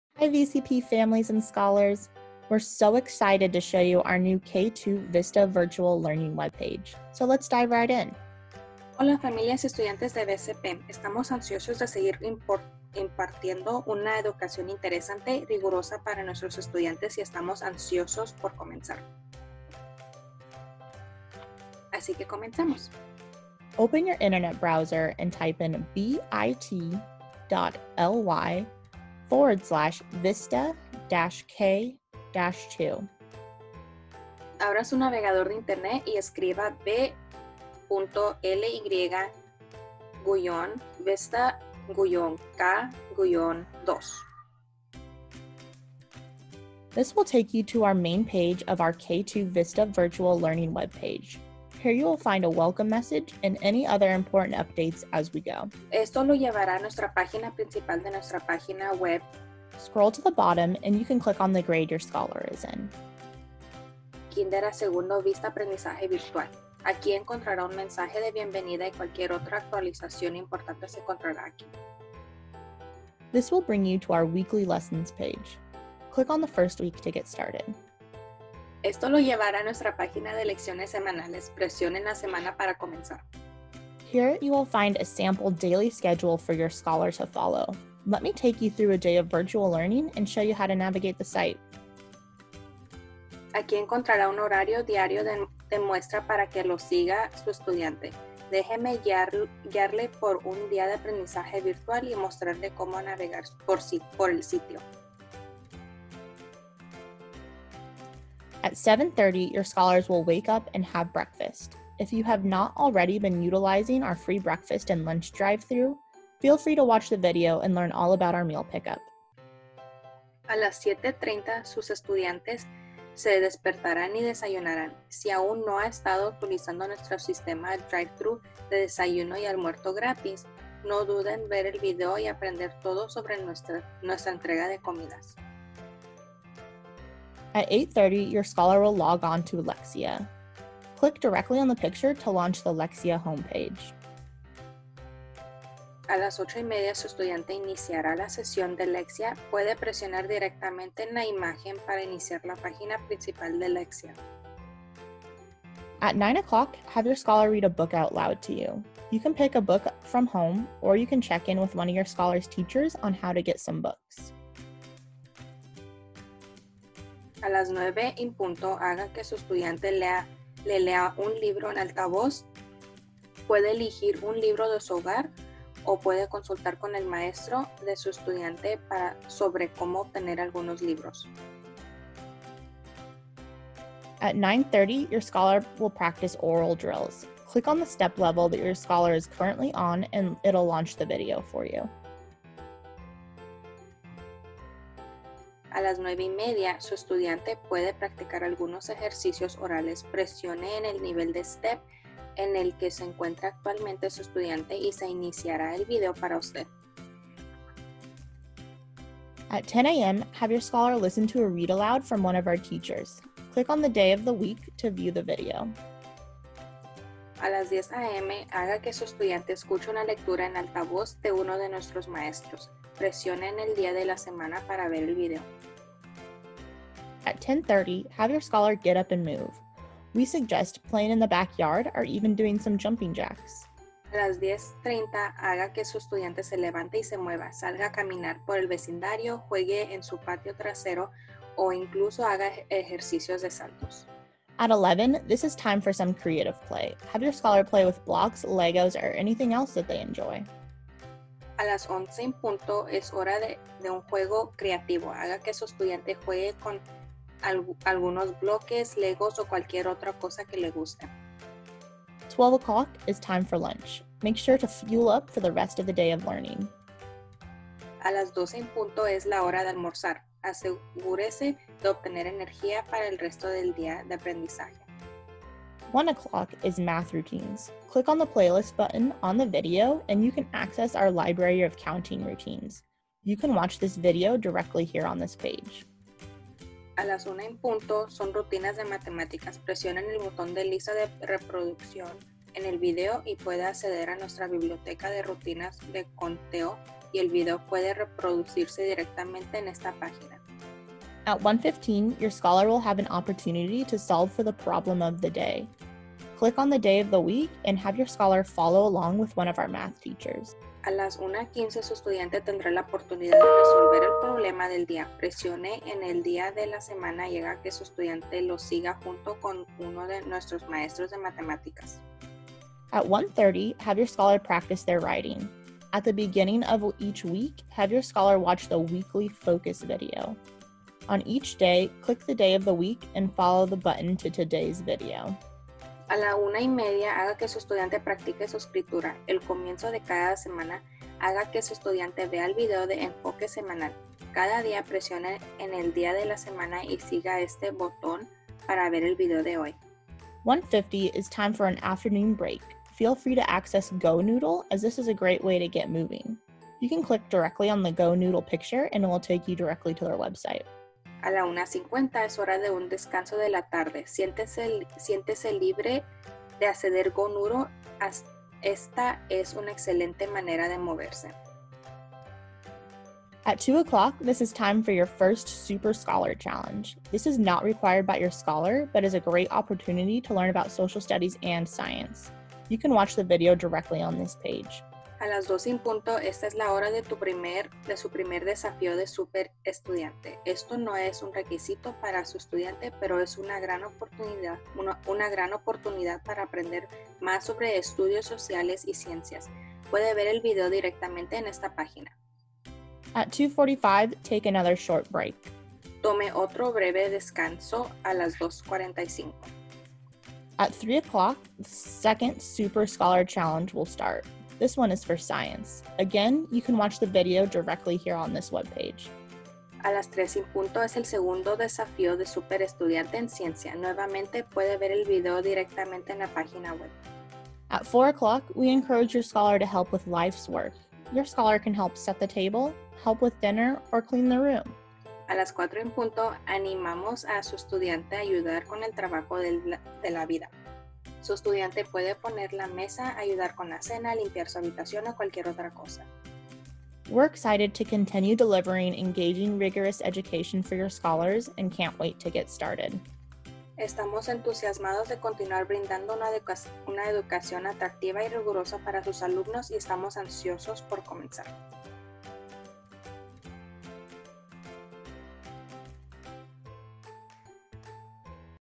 Logging on to Vista Virtual (Grades K-2)
Watch this video to learn how to access and navigate to VCP's K-2 Virtual Learning.